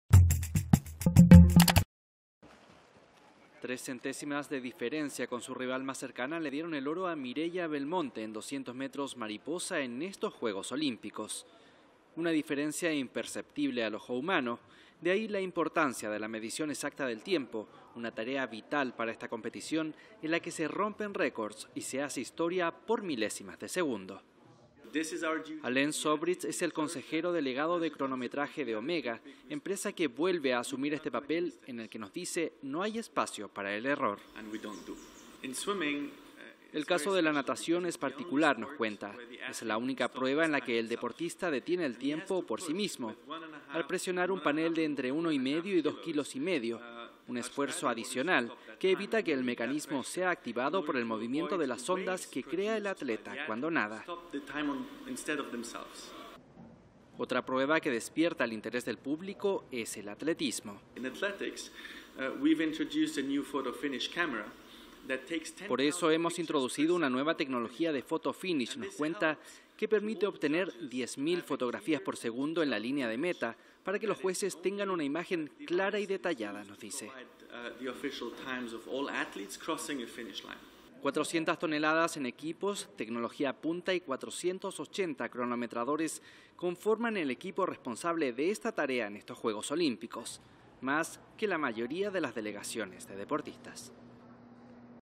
Cronometraje en los JJOO: el oro por una milésima de segundo
Río de Janeiro, 11 ago (EFE), (Imagen: Juan Yagüe).- La medición exacta del tiempo es uno de los elementos claves del éxito de los Juegos de Río. Así lo sostiene Alain Zobrist, Consejero delegado de cronometraje de Omega, compañía que vuelve a asumir el papel de "cronometradora oficial" de los Juegos Olímpicos, una tarea de vital importancia para esta competición en la que se rompen récords y se hace historia por milésimas de segundo. Palabras clave: Deporte-Eventos deportivos-Juegos olímpicos de verano